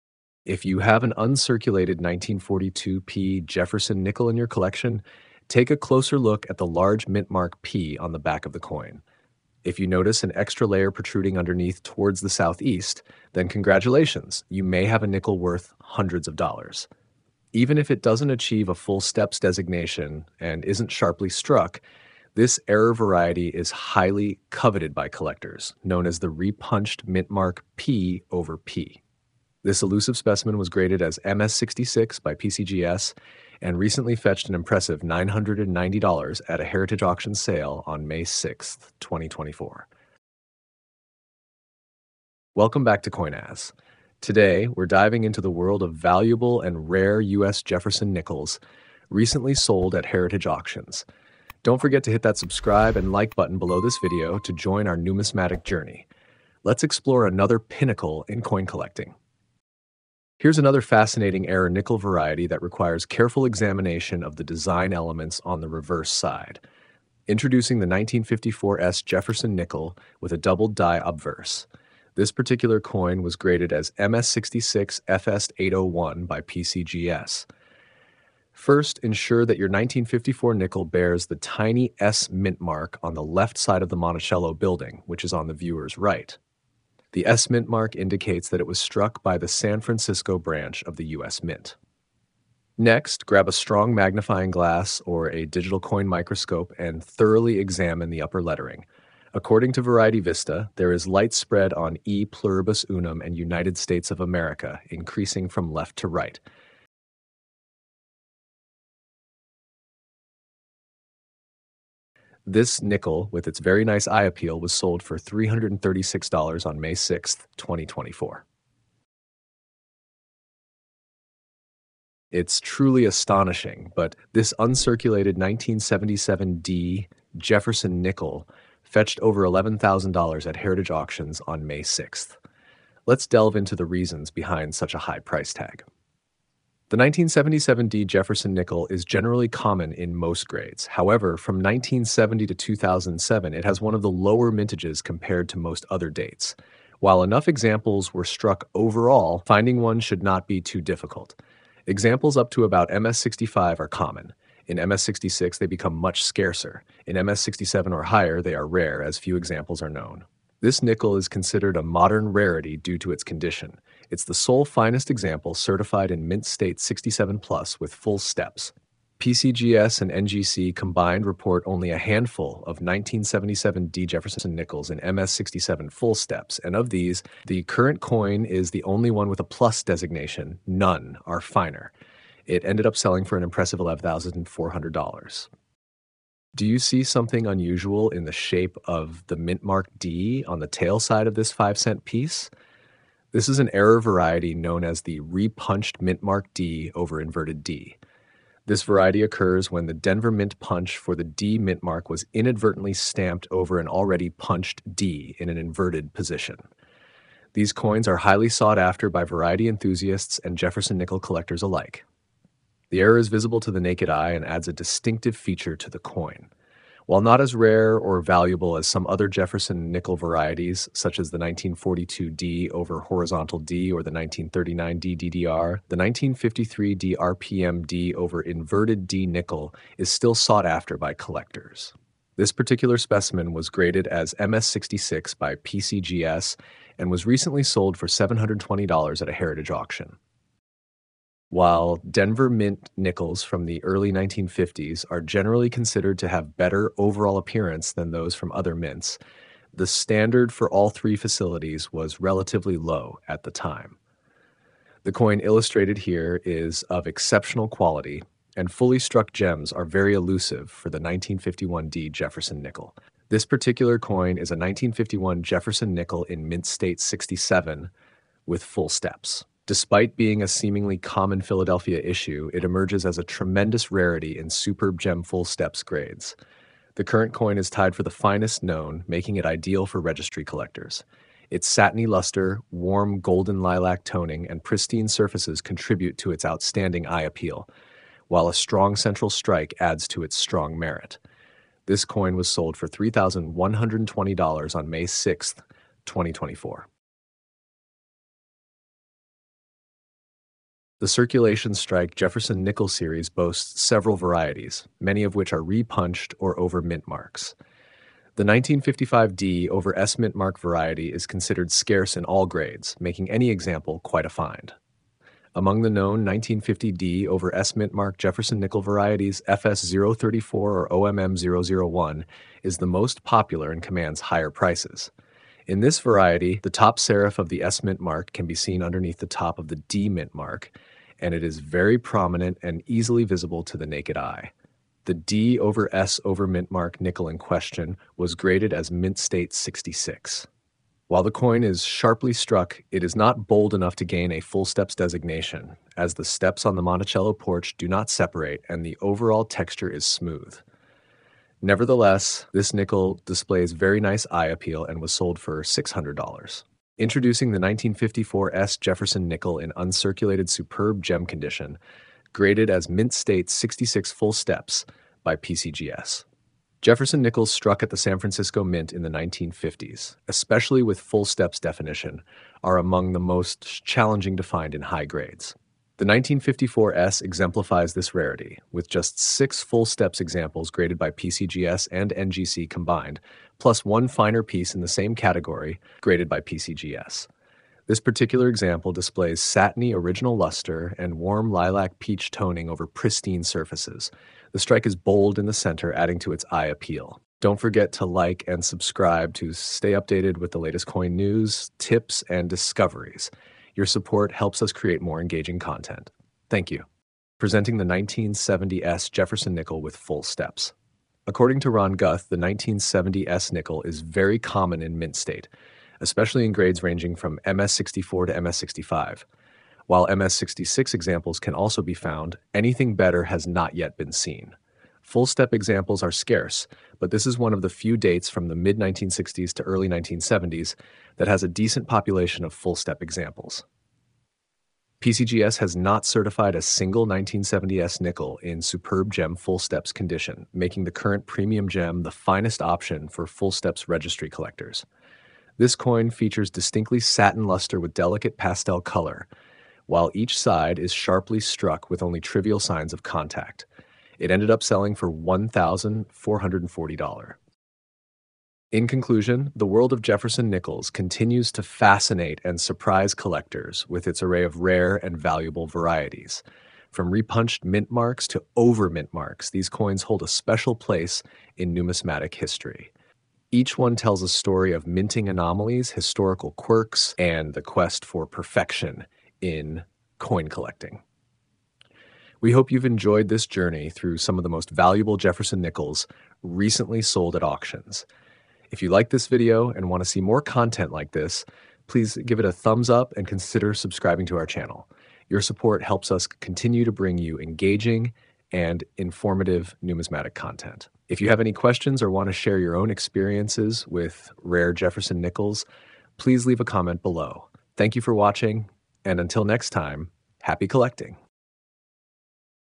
Rare Jefferson Nickels Unveiled: Valuable US Coins You Should Keep!
In this video, we dive into the world of rare Jefferson nickels, uncovering valuable coins that every collector should keep an eye out for. From repunched mint marks to over mint marks, these coins hold significant value and historical interest. Learn how to identify these rare gems and discover their worth in today's market. Join us as we unveil the secrets of valuable US coins that belong in your collection!